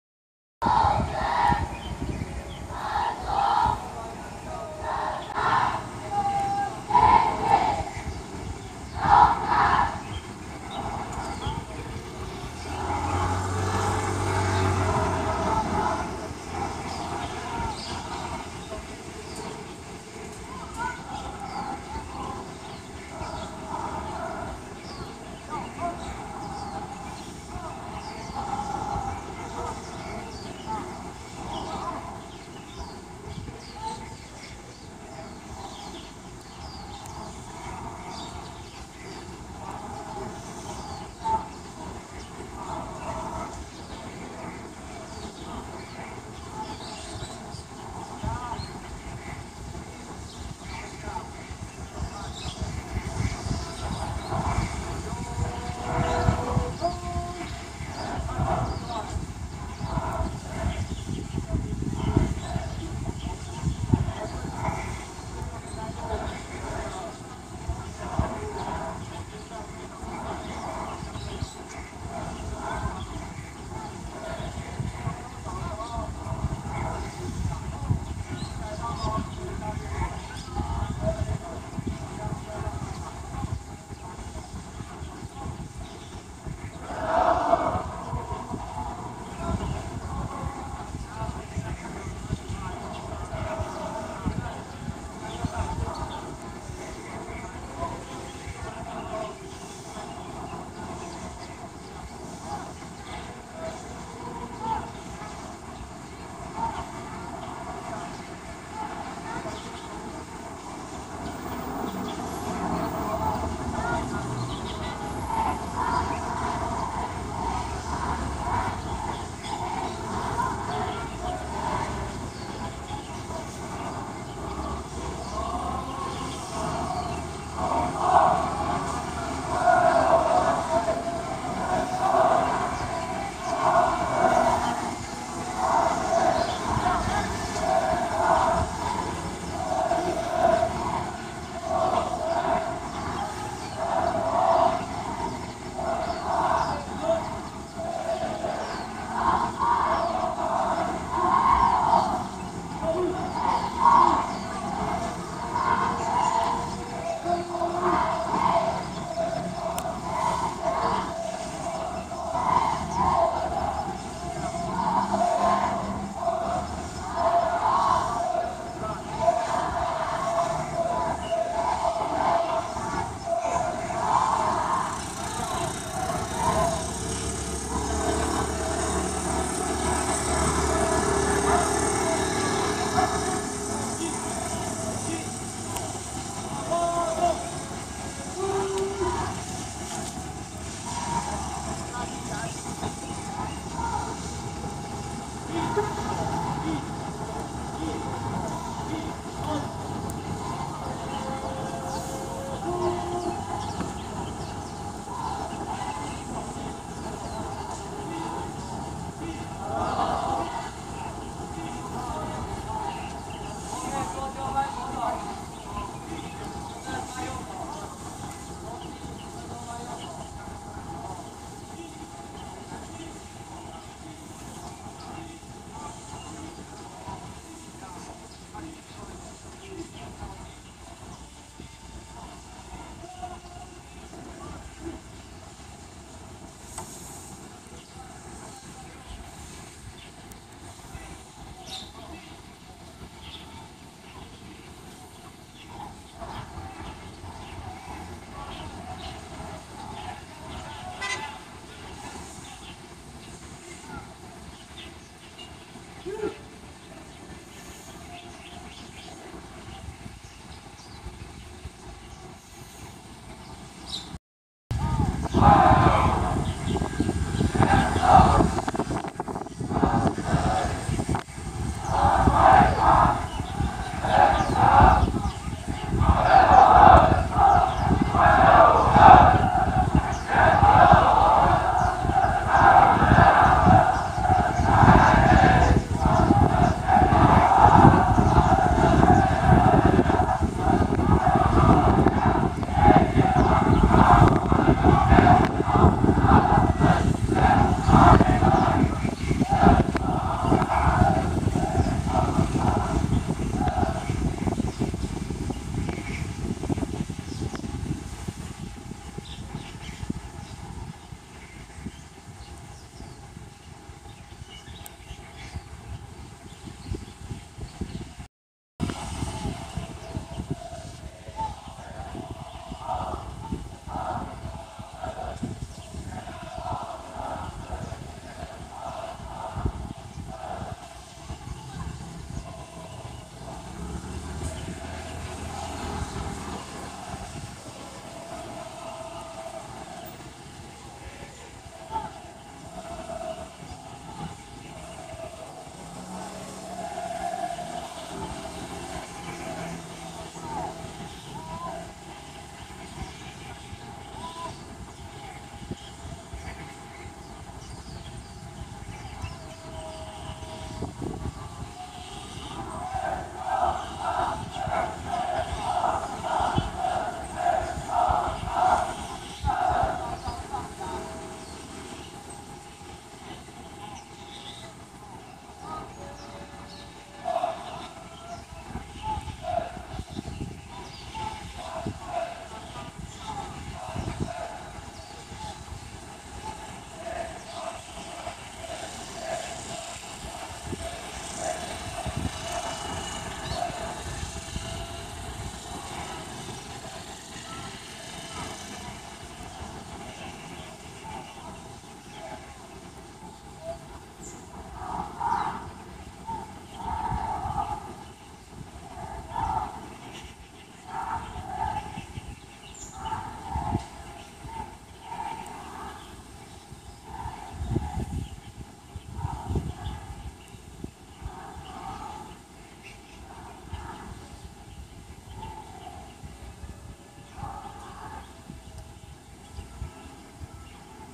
熊貓老爹：2016/08/06上午 入伍生團「離開中正堂」
圖文影片彙整於「神仙、老虎、狗 」部落格
「從」寶可夢科技遊戲， 「到」入伍生團掌旗兵預習結訓閱兵， 「談」陣雨預報 。